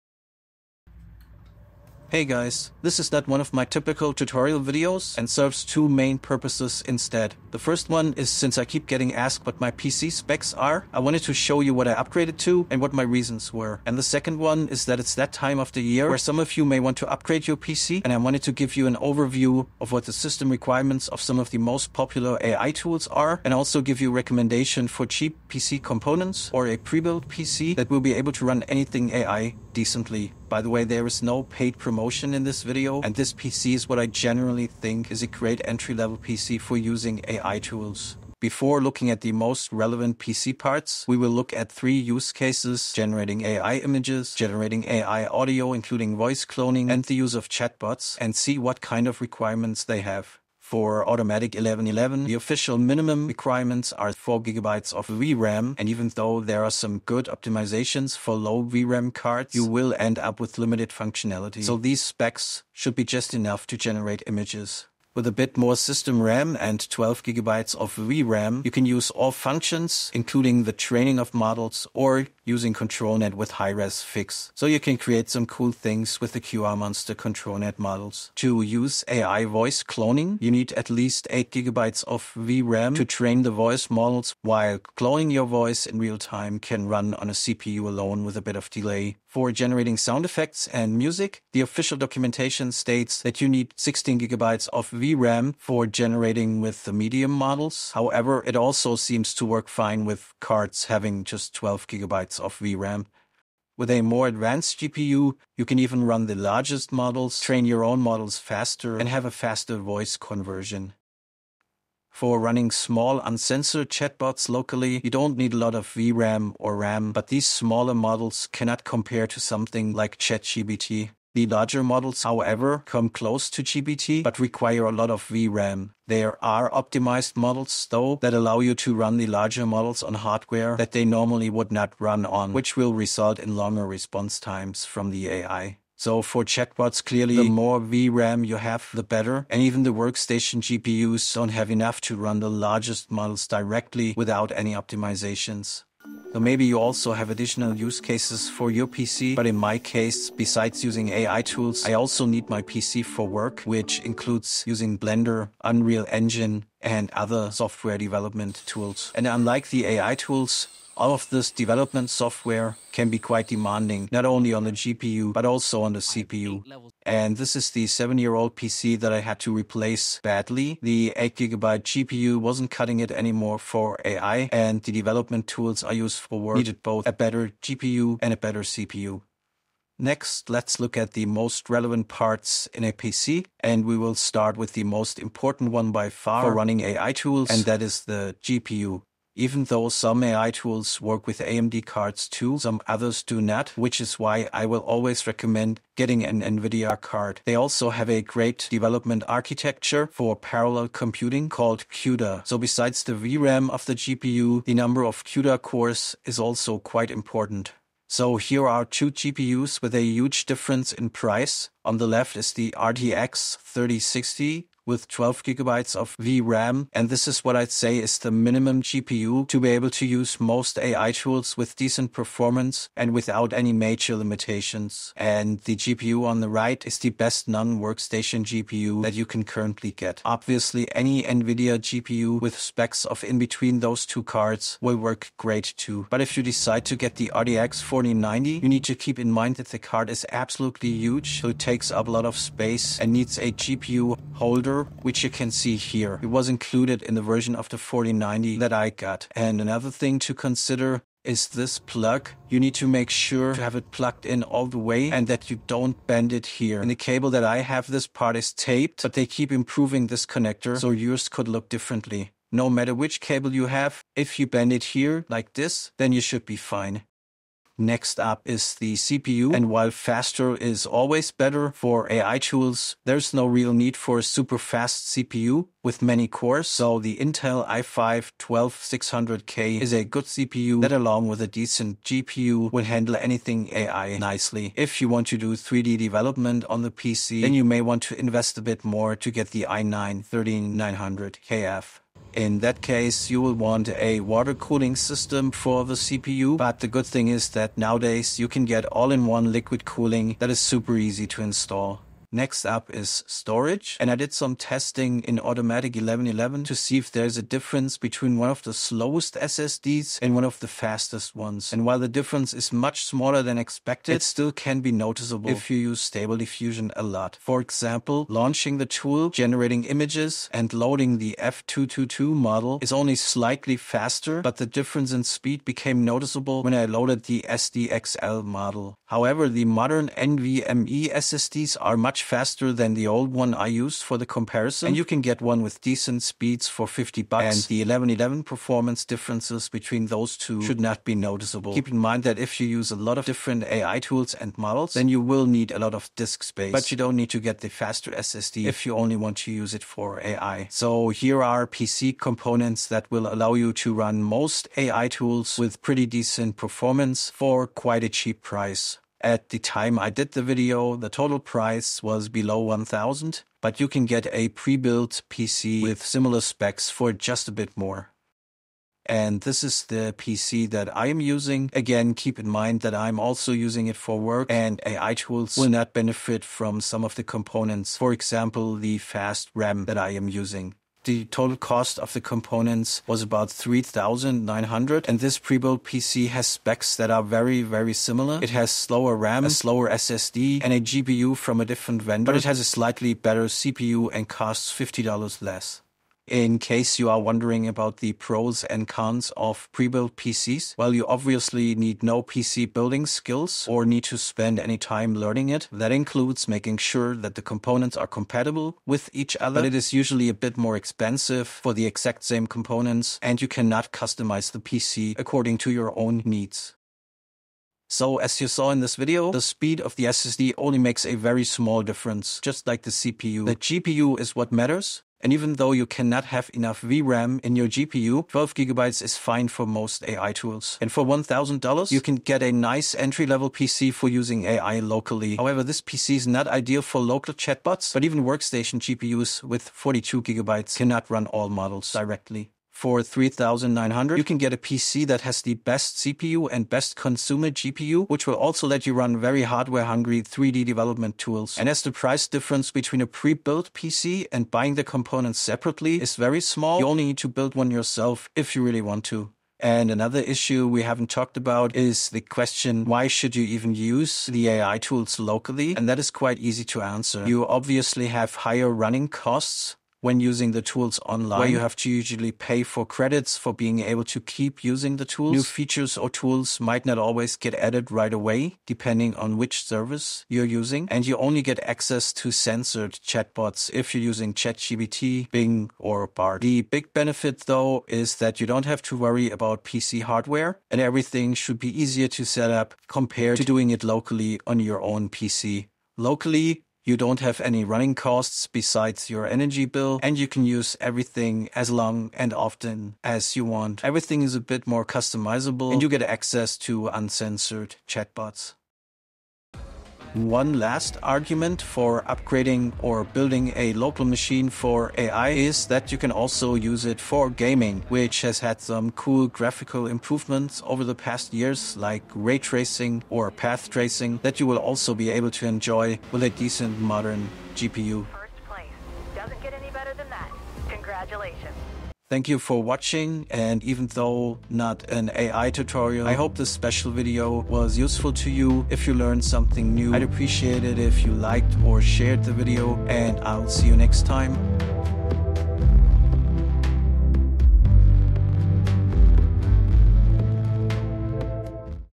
PC Hardware Upgrade For Running AI Tools Locally
With the holiday season approaching, this video summarizes the system requirements of popular AI tools to run them locally and presents a recommendation for an entry level PC (separate components or prebuilt) that can run most AI tools without limited functionality. I also show you which PC I am using and give the reasoning why I needed the specific hardware.

/UPDATE
It is now November 2024 and nothing much has changed since I made this video. Intel is about to release a new range of CPUs but as we know, Ai is all about the GPU and VRAM. NVIDIA is going to release their new range of GPUs early in 2025 . Depending on the price of these new GPUs, we might see a decrease in price of the current models but this is always hard to predict.

Chapters:
00:00 Intro
00:50 Stable Diffusion System Requirements
01:19 AI Audio System Requirements
01:52 AI Chatbots System Requirements
02:26 Other Use Cases
03:00 GPUs for AI tools
04:28 CPUs for AI tools
05:02 Storage for AI tools
05:50 Entry Level PC Components
06:03 Prebuilt Entry Level PC
06:09 Premium PC Components
06:25 Prebuilt Premium PC
06:37 Pros and Cons of prebuilt PCs
06:57 Summary
07:39 Pros and Cons of using AI tools locally
08:30 Gaming